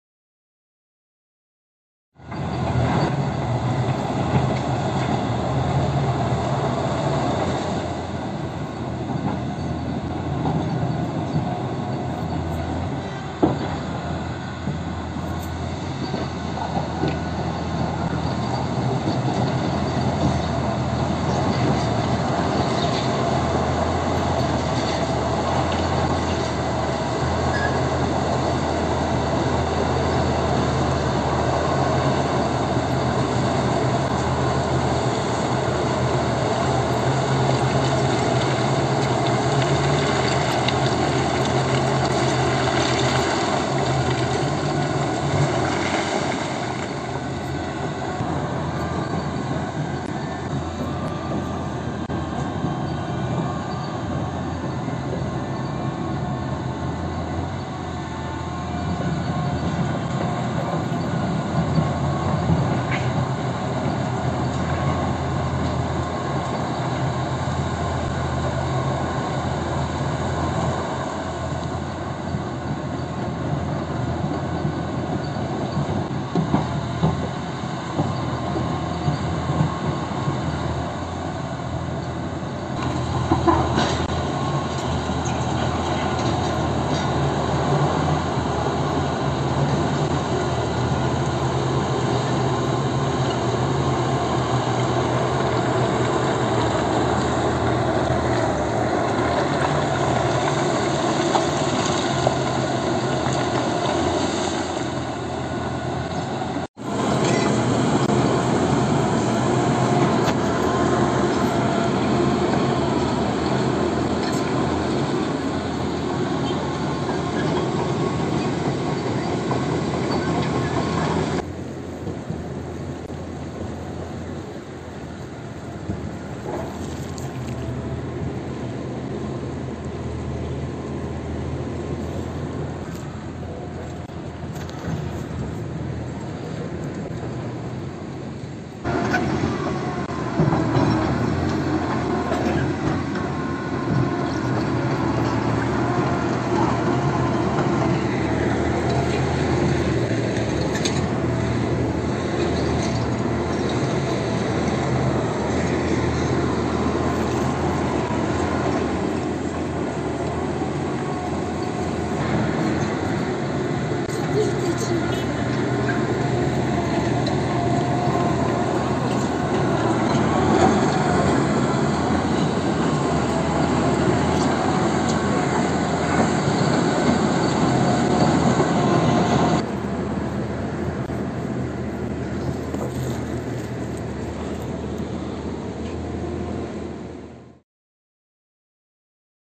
Bayburt'ta Çoruh Nehri yatağında temizlik başlatıldı - 24/10/2022
Bayburt Medya Com